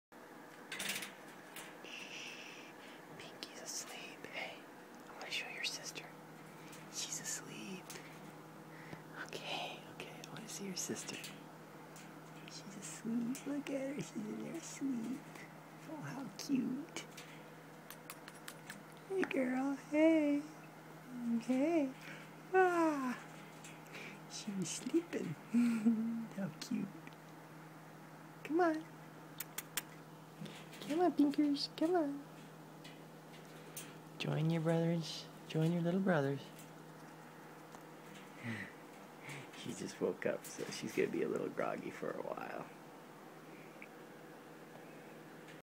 Pinky Yawns
Pinky yawns while I was wakeing up the kids up for breakfast.  They had Cherry tomatoes, lettuce, broccoli and almonds.